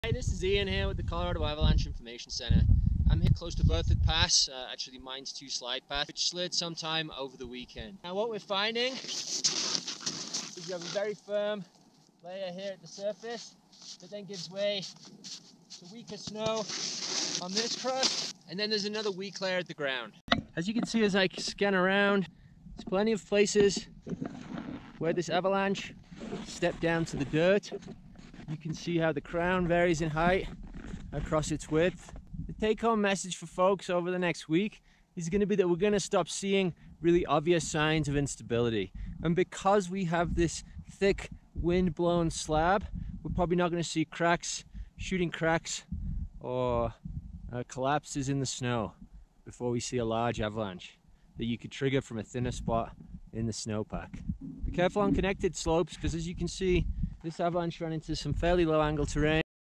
December 2, 2024 - Conditions Update from Berthoud Pass area
Reports of shooting cracks and collapsing sounds in the snow are slowing down, but don’t let the lack of obvious instability signs lull you into a false sense of security. Observers are noting fewer avalanches, and they’re becoming harder to trigger. However, the recent storm created a deep, well-connected snowpack in many areas, increasing the potential for larger avalanches. You can still trigger avalanches large enough to bury or kill a person. Unless a slope already slid during the natural avalanche cycle, treat all steep slopes with a Persistent Slab avalanche problem as guilty.
➡️ Always check the latest avalanche forecast @ Colorado.gov/avalanche.